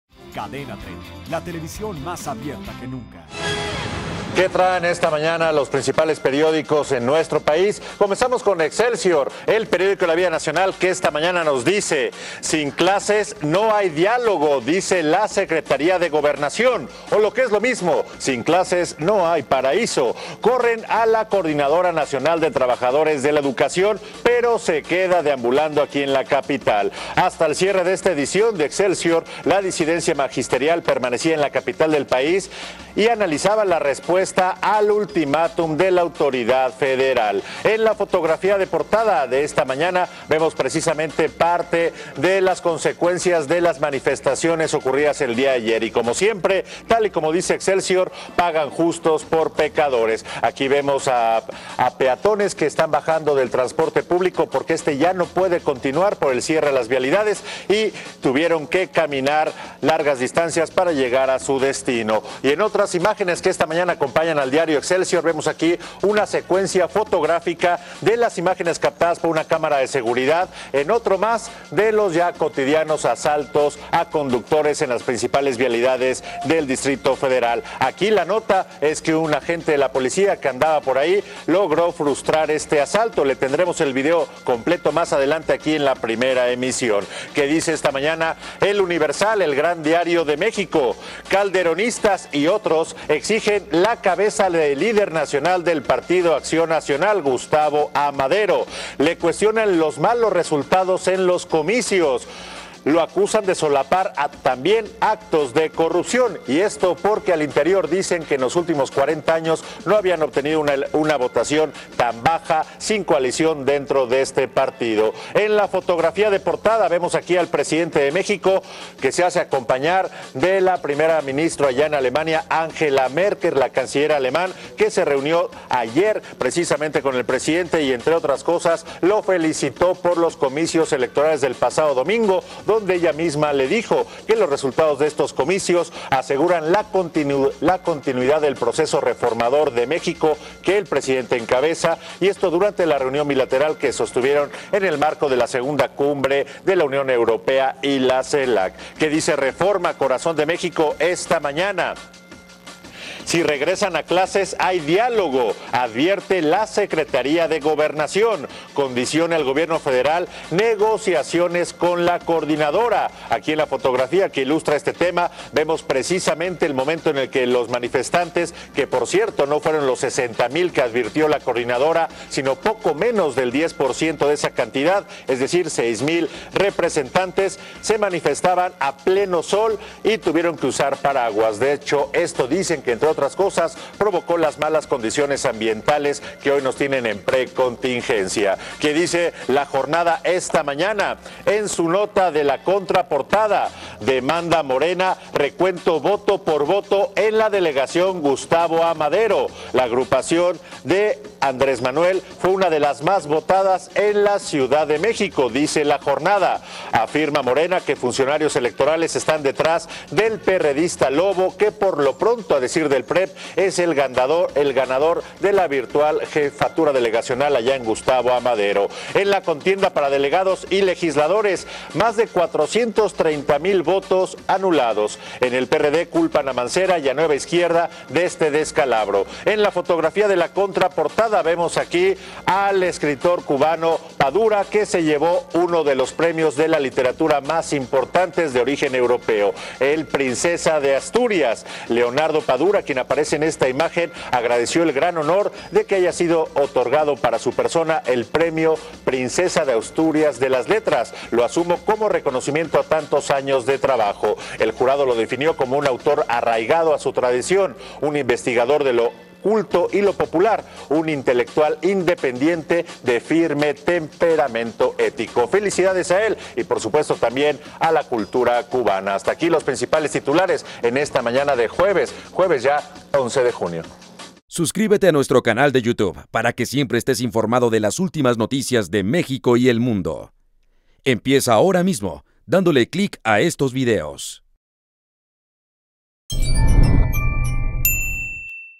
Así amanecieron los principales diarios de México hoy 11 de junio
Así lucen las portadas de los principales diarios del país.
11 de junio 2015.